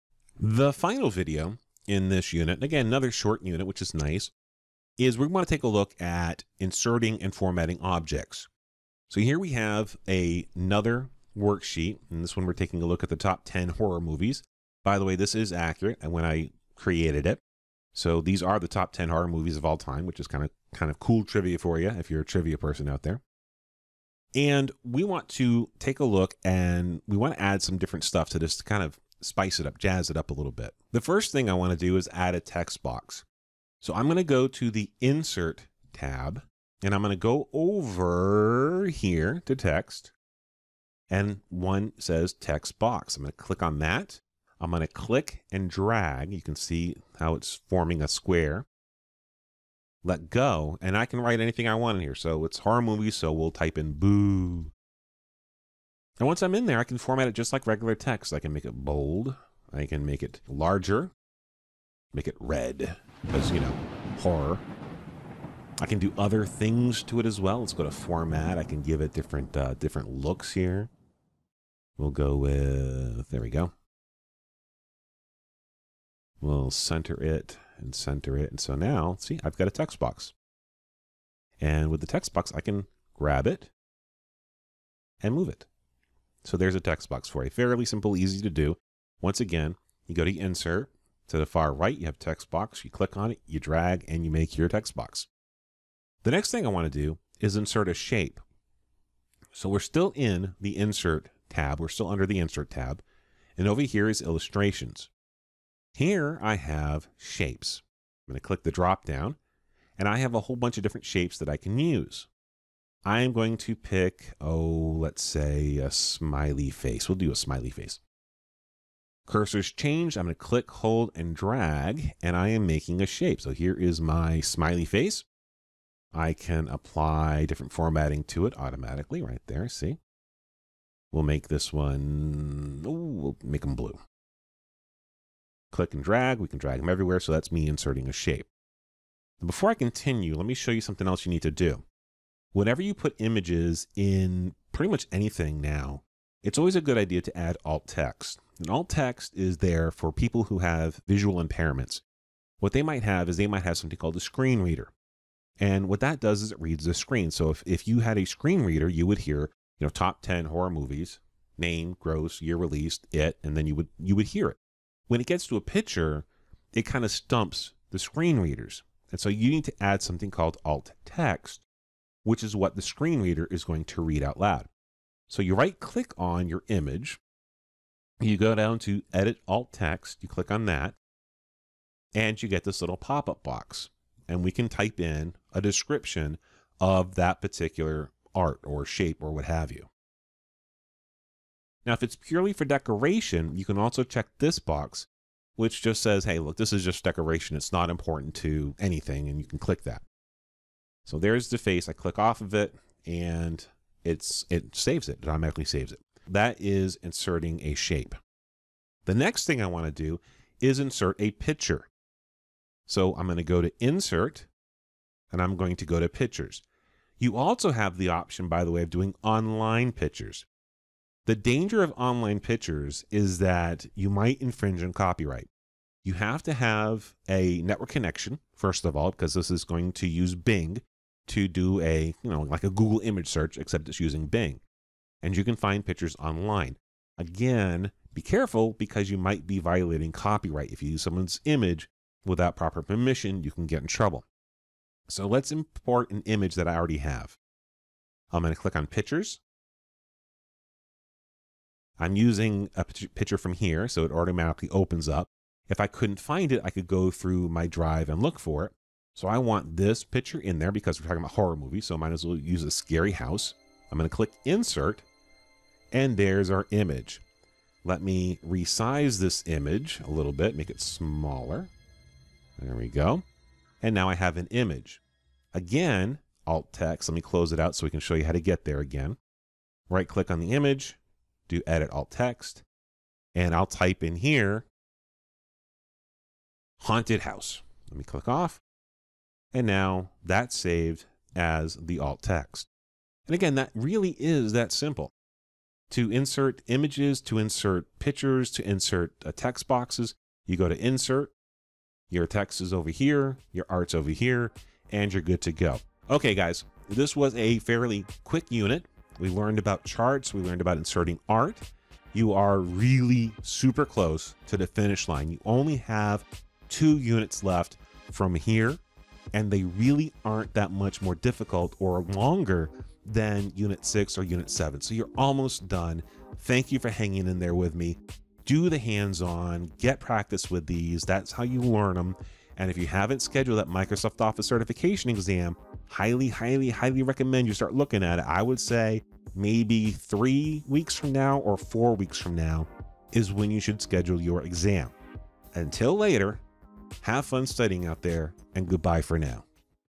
Insert and Format Objects (07.07) - Microsoft Excel: From Beginner to Certified Professional
Our final lesson in this unit introduces us to inserting and working with different objects.

When you have completed this lesson, you will be able to insert text boxes, add shapes, locate and use images, and add alt. text to your visual elements to help those with visual challenges.